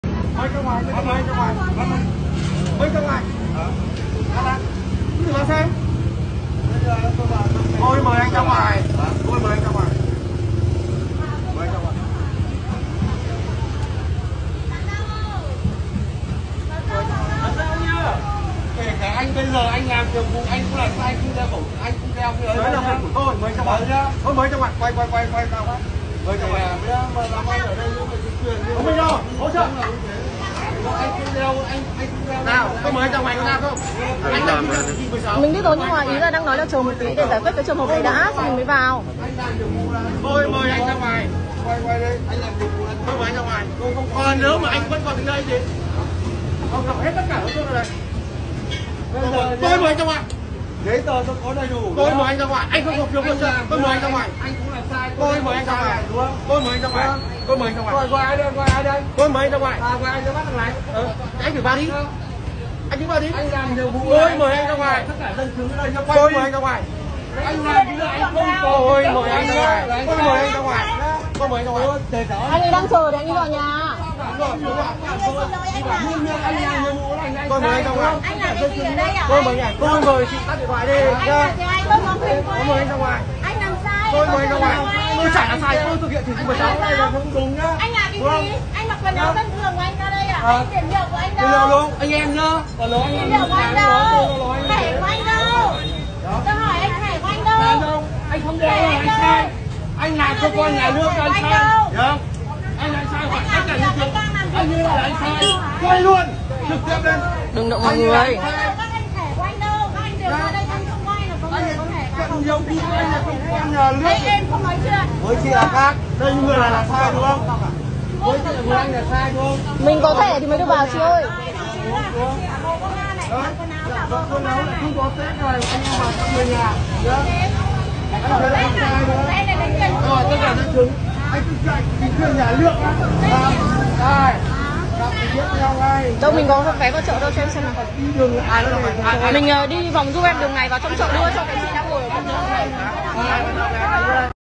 Thấy ngán mấy ông bố đời ghê cả vợ lẩn chồng đang dịch mà
Thấy ngán mấy ông bố đời ghê cả vợ lẩn chồng đang dịch mà rồng rắn cứ muốn leo lên đầu người khác.